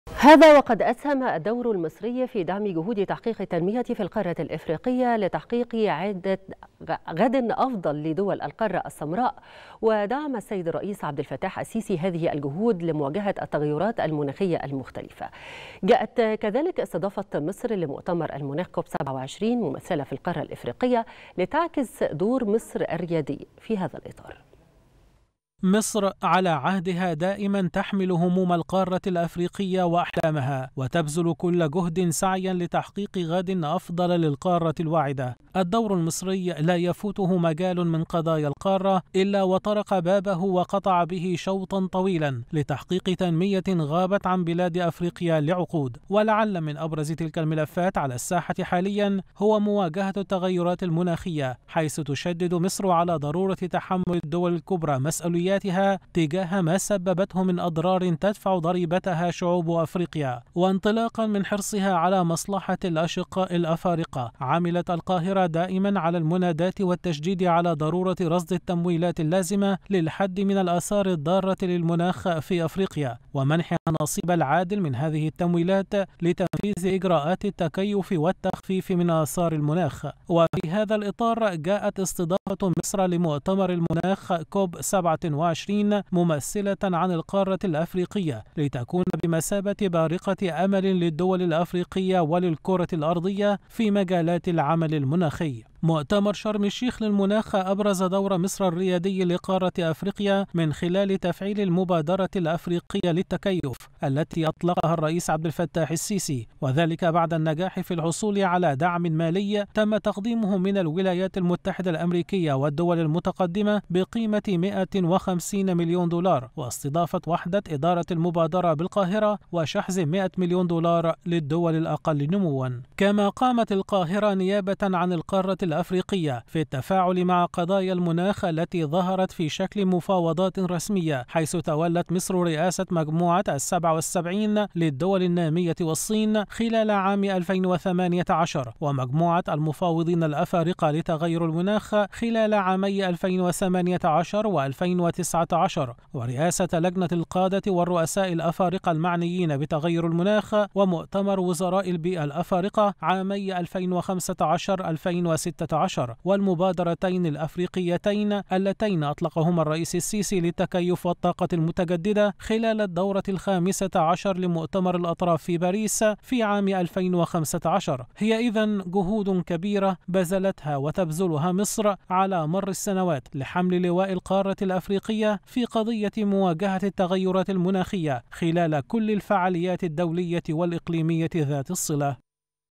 مصر على عهدها دائما تحمل هموم القارة الأفريقية وتبذل كل جهدٍ سعيا لتحقيق غداً أفضل للقارة الواعدة
تغطية خاصة | مصر على عهدها دائما تحمل هموم القارة الأفريقية وتبذل كل جهدٍ سعيا لتحقيق غداً أفضل للقارة الواعدة